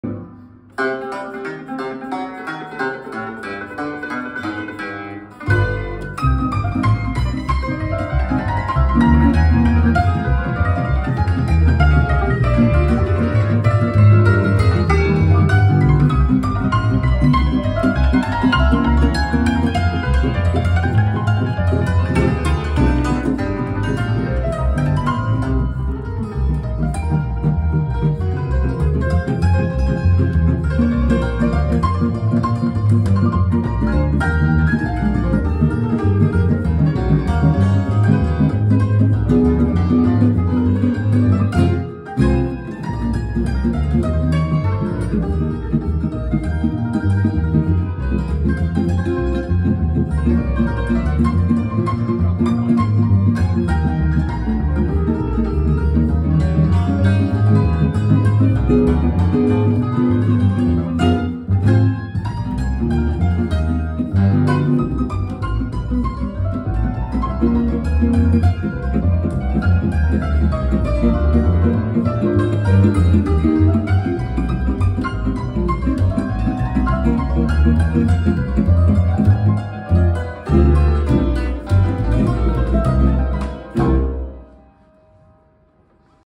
Farkas Rózsa (cimbalom)
Három zseniális muzsikus: Farkas Rózsa – cimbalom,  György Mihály – gitár, Novák Csaba – nagybőgő.

A felvételen “csak” egy hangbeálló próba az Óbudai Társaskörben, 2023. július 15-én, este 7-kor.